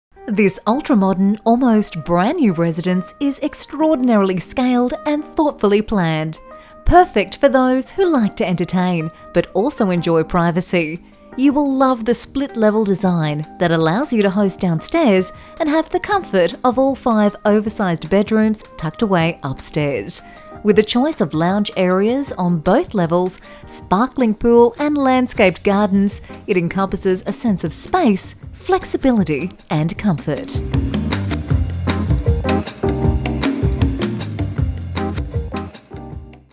54 Tranters Avenue, Camp Hill :: Place Estate Agents | Brisbane Real Estate For Sale
The Ultimate Luxury Family Home

Presenting the very best in contemporary additions, this ultra modern and almost brand new, hardly lived in residence is extraordinarily scaled and thoughtfully planned.

Perfect for those who like to entertain but also enjoy privacy, you will love the split level design that allows you to host downstairs and have the comfort of all five bedrooms tucked away upstairs.

With a choice of lounge areas on both levels, sparkling pool and landscaped gardens it encompasses a sense of space, flexibility and comfort.

With soaring high ceilings and clean white tones, this exclusive home boasts stunning large tiles and beautiful open plan living and dining design.

Relax in the downstairs living room, or entertain with ease as you open the sliding bi-fold doors and enjoy blissful moments on the terrace overlooking the glittering swimming pool, grassed yard and beautiful leafy outlook.

Alfresco dining awaits you with a designated outdoor kitchen with a barbeque plus plenty of bench space to prepare your meals. The chef in the family will appreciate the main kitchen as well which features stunning cabinetry with ample storage, gas cooktop, beautiful white stone benchtops, floating island bench and breakfast bar and an extra large walk in pantry.

Spend time with the family relishing the spectacular and specifically designed media room with darkened walls and soundproofing, for the ultimate in media enjoyment.

Finishing off the ground level is the office serviced by a powder room. Five generously sized bedrooms are strategically positioned upstairs,
the master is particularly grand in size with beautifully finished ensuite with double basin vanity, separate toilet, large shower and an oversized walk in robe with full cabinetry.

Positioned next to the master is bedroom 2 with a bathroom, while bedroom 3, 4 & 5 also have a bathroom to share at the front of the home.

A big lounge area awaits you upstairs, a perfect teenagers retreat with multi functional layout and extra storage.

Included in this premiere family property, many features include; Gas hot water, vacuum-aid, lock up accommodation for two cars, alarm and intercom system, generous laundry area and ducted air conditioning throughout. This family residence is immaculate and located in a quiet street surrounded by quality family homes. Ideally positioned close to plenty of amenities, quality schools, public transport options, parks and sporting fields, Westfield Carindale Shopping Centre and only 9km to Brisbane CBD, this one is well deserving of an inspection.